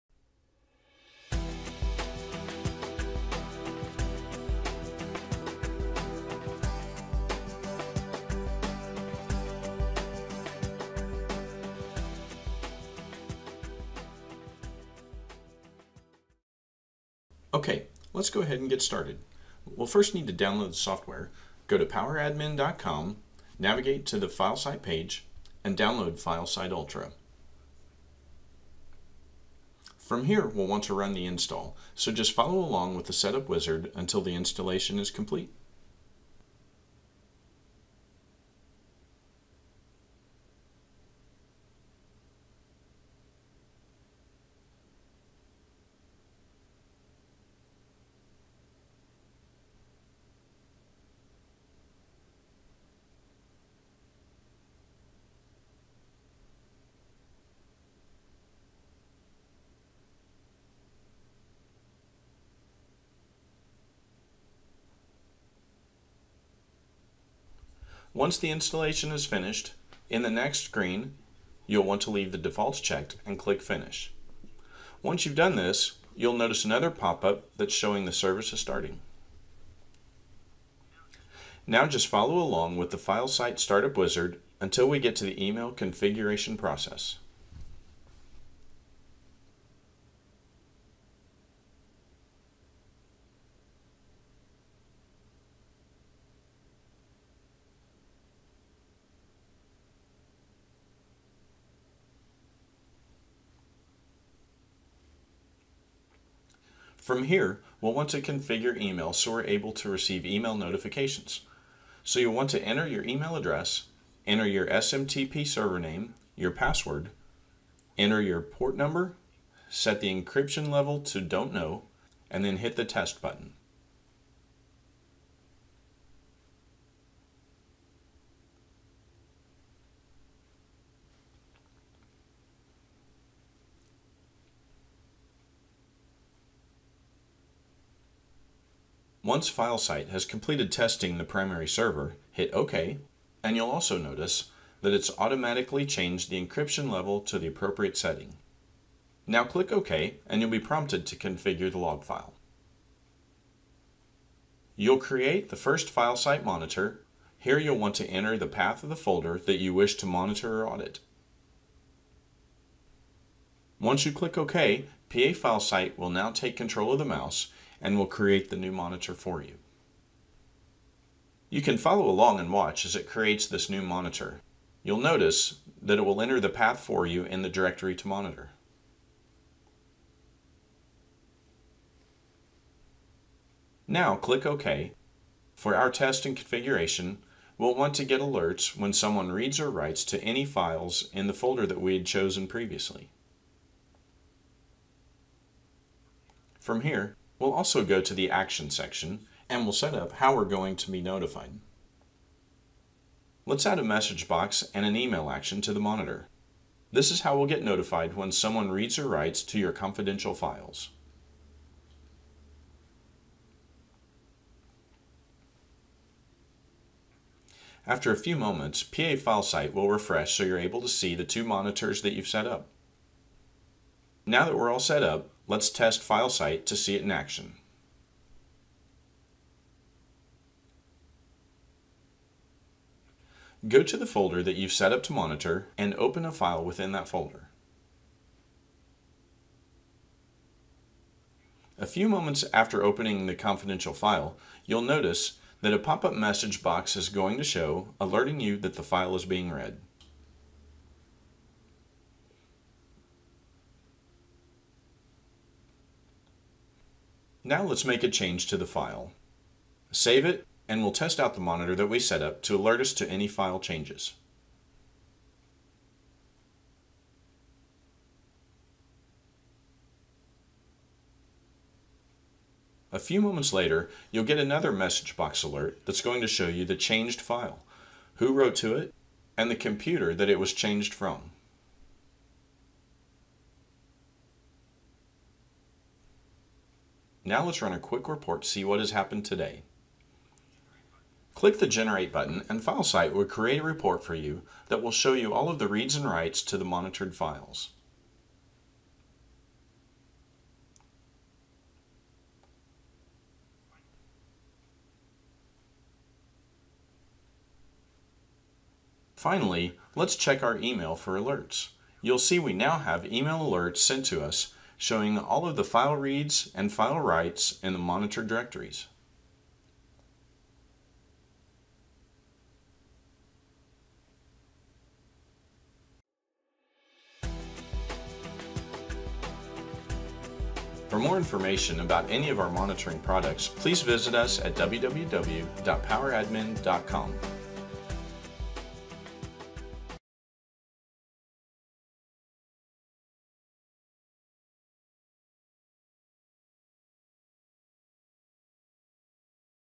File Monitoring Software Program for File Auditing - PA File Sight
Follow along as we download the file auditing and file monitoring software program, PA File Sight. You can install and configure and have it up and running, receiving alerts and creating reports...all in less than 5 minutes!

PA File Sight is popular among companies that are concerned with HIPAA and PCI compliance, internal corporate compliance, or just want to know who deleted a file. This software enables you to see:

   * Who is reading from and writing to important files
   *  When new files or folders are created
   * Renaming of files or folders
   * When files or folders get deleted
   * Who did it and often what program they used

PA File Sight's active file and folder auditing can be an important part of meeting regulatory standards such as Sarbanes-Oxley, ISO and the Payment Card Industry Data Security Standard. With the built in security settings, managers will know if files are tampered with, deleted, or being read by those who shouldn't have access.

PA File Sight's advanced auditing technology has virtually no performance impact, unlike solutions that require native auditing. See for yourself why companies around the globe choose PA File Sight as their file auditing software of choice.

Download the trial today, it will be the best trial software you have ever tried. :-)